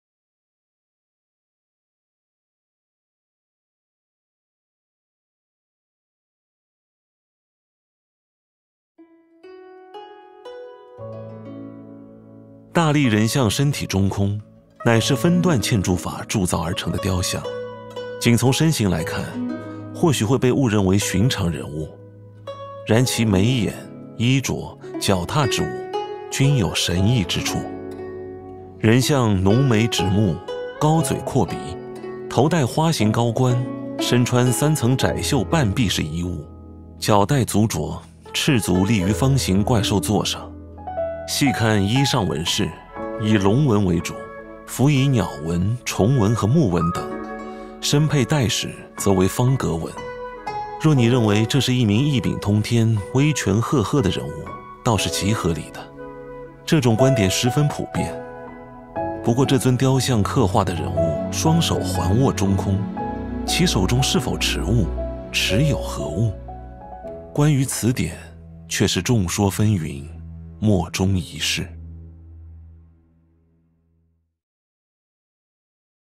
Mysterious Docent at the Sanxingdui Tells You the Secret of the Bronze Standing Figure
Does the bronze standing figure from Sanxingdui look like a authoritative person? What is it holding in its hand? The mysterious docent of Sanxingdui tells you the secret of it.